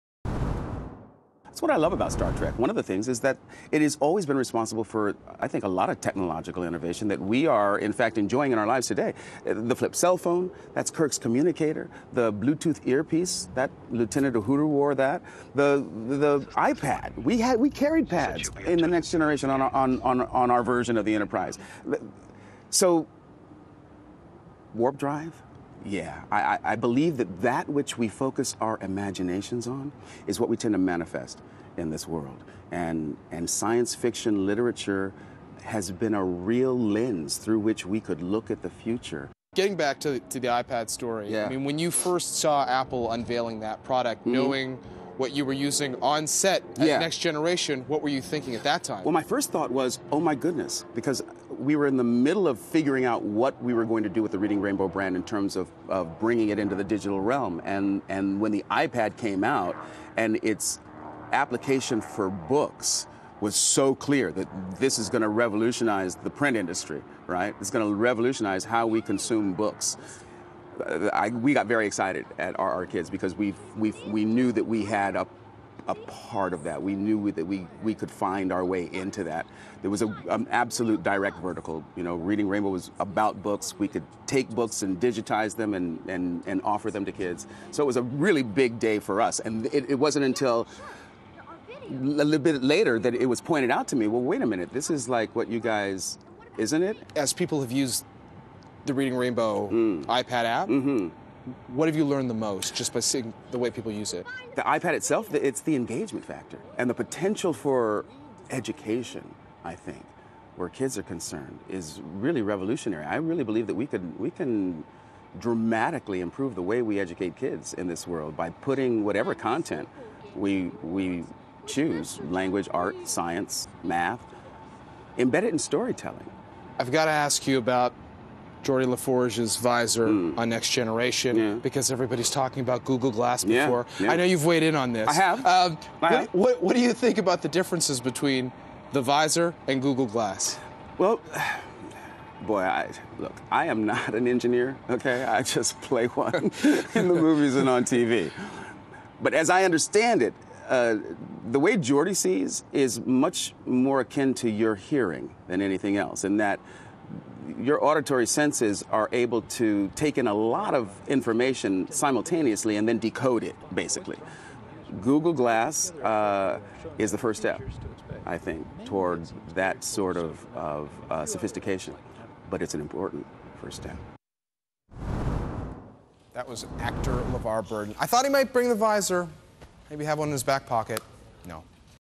'Star Trek' Used iPad-Like Tablets: Levar Burton
March 13 (Bloomberg) -- Actor Levar Burton explains how Star Trek is responsible for a lot of the technological innovations that we enjoy today. He speaks with Jon Erlichman on Bloomberg Television's "Bloomberg West." (Source: Bloomberg)